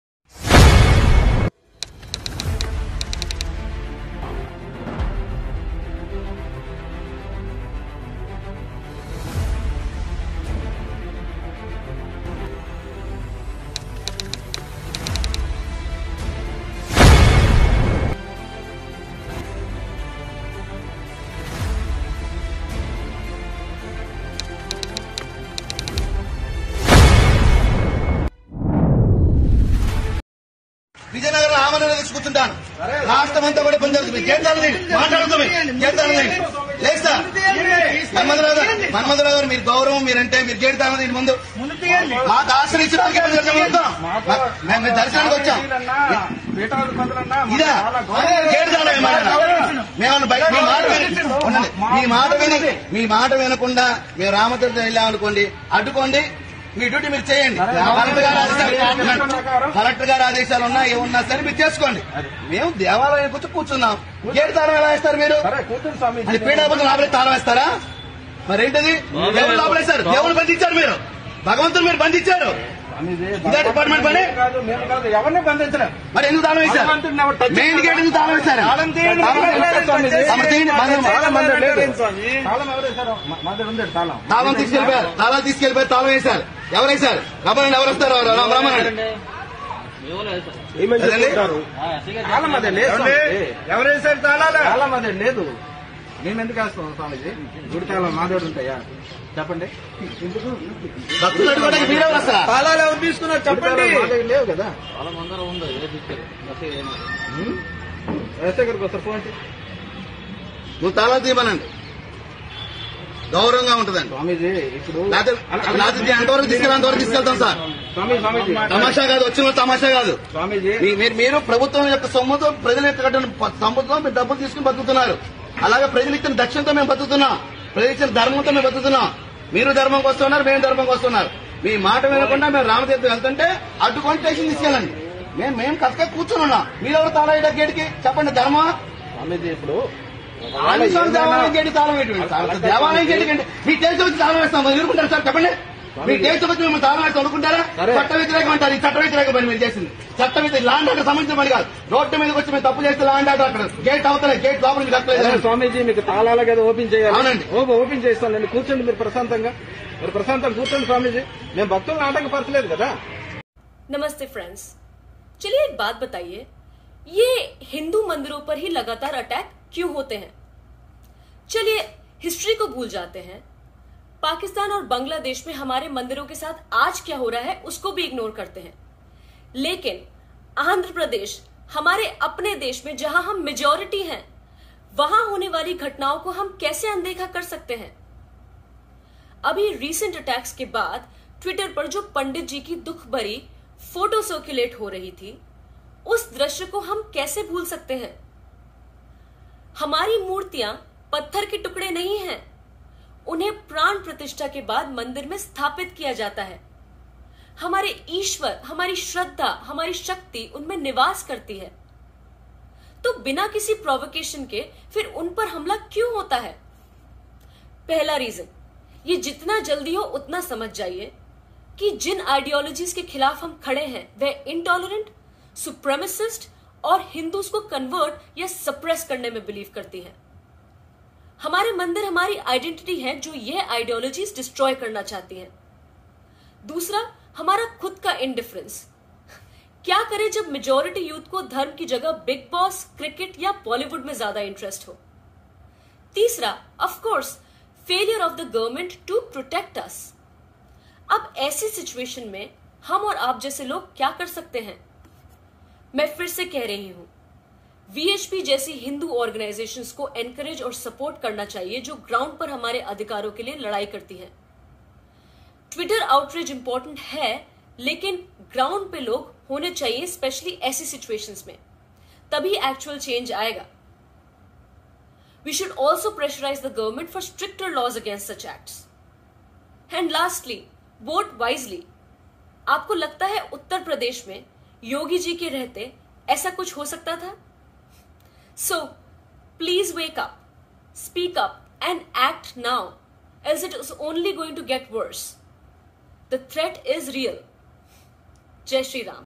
Why are Hindu Temples under constant attack?
yaha apko kuch bhi milega linken jo bhi milega woh mazedaar hoga

Police put a lock on the entrace gate of temple, Swamijis are locked inside the temple. Hindus are not even allowed to protest in Christian raj. So far 128 incidents of temple vandalism happened but not a single culprit was arrested. What is going